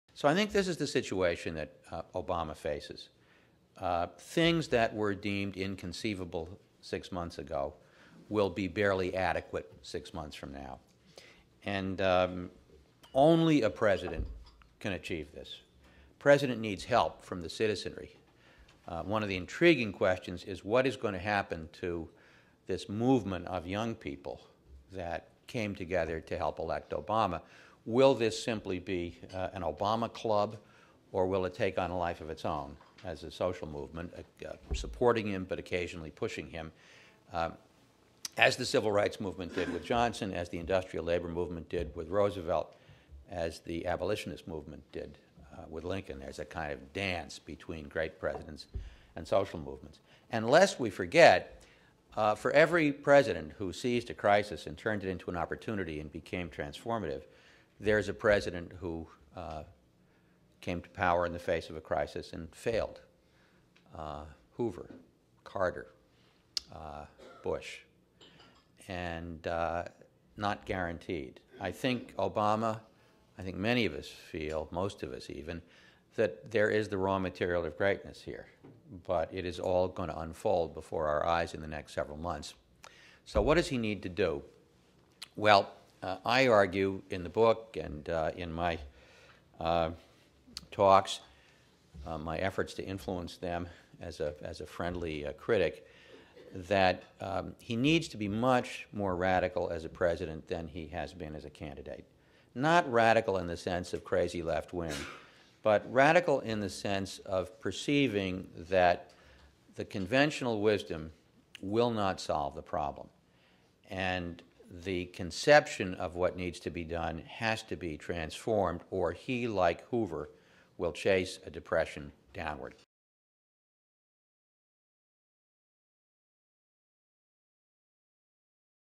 Robert Kuttner: What Obama Faces
"So I think this is the situation that Obama faces. Things that were deemed inconceivable six months ago will be barely adequate six months from now," says Kuttner.
He continues, "Lest we forget, for every president who seized a crisis and turned it into an opportunity and became transformative, there's a president who came to power in the face of a crisis and failed—Hoover, Carter, Bush."
This Carnegie Council event took place on November 12, 2008. For the full video, audio, and transcript, go